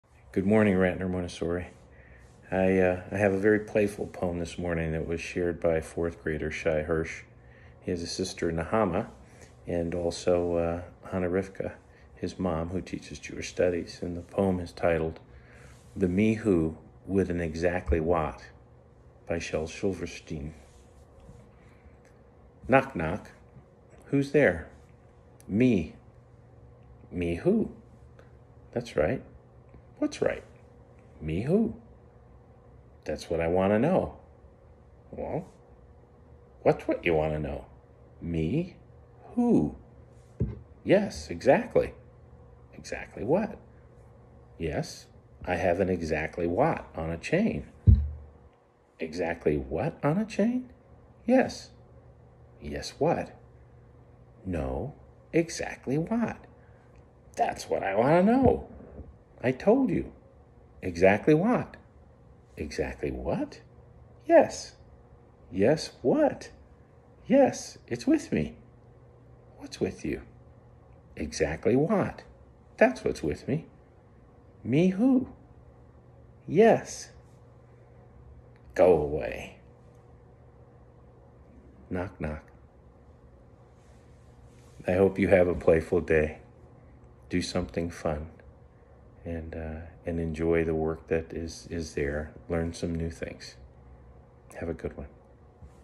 Hello from Mike Griffith April 7 2020
In today's morning greeting, Mike shares a poem suggested by Ratner students, and reminds us to be playful!